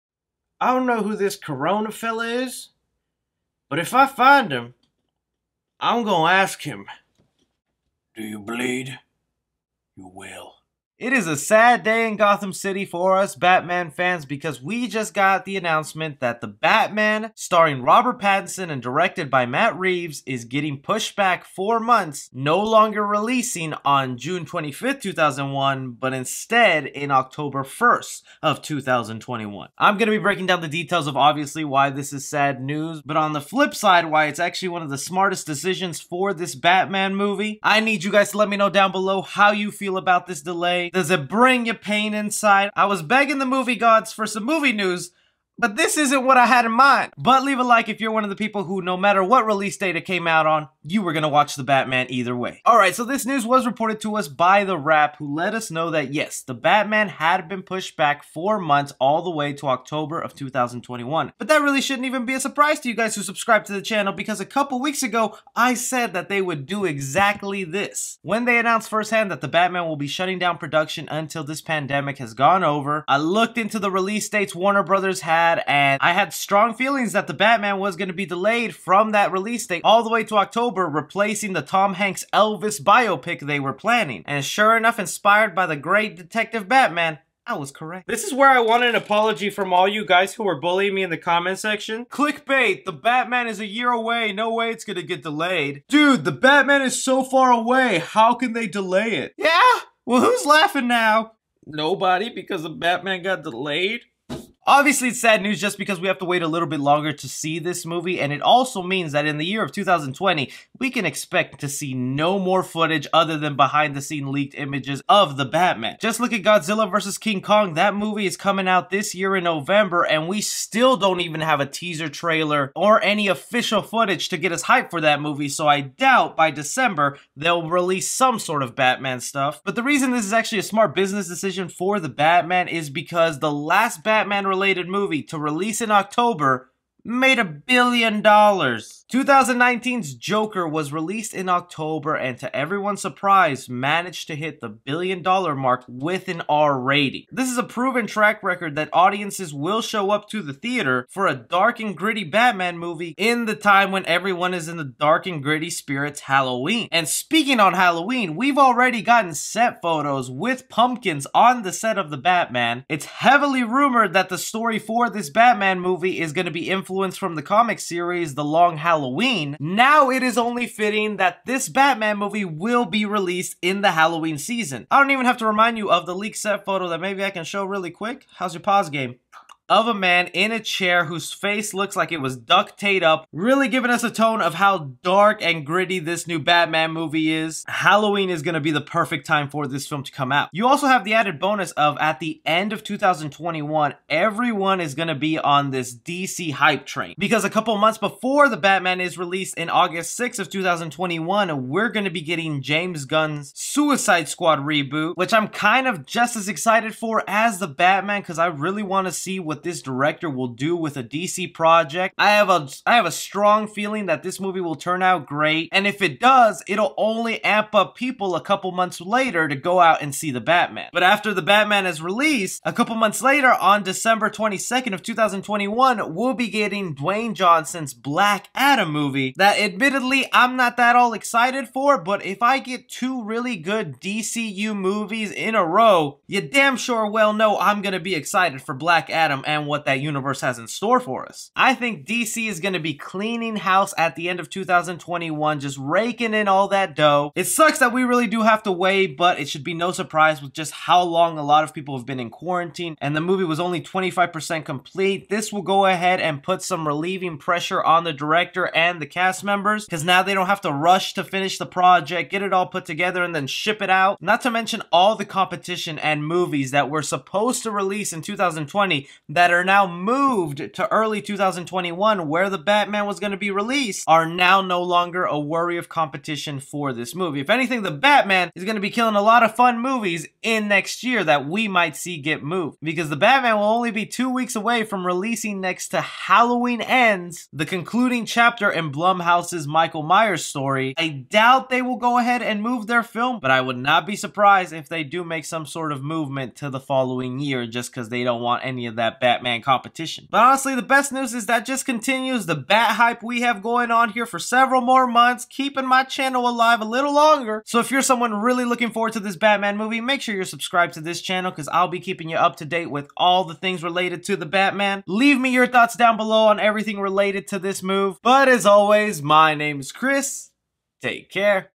The Batman (2021) Sad News & MORE!!
The Batman 2021 Delayed From June 2021 to October Of 2021. The Batman movie directed by Matt Reeves & starring Robert Pattinson batman. very sad news but Warner brother wants to redo the Joker 2019 box office for the New Batman 2021. No The Batman 2021 Trailer in 2020 lets hope for a better batman update soon. no batman leaks images or new footage

SOCIAL MEDIA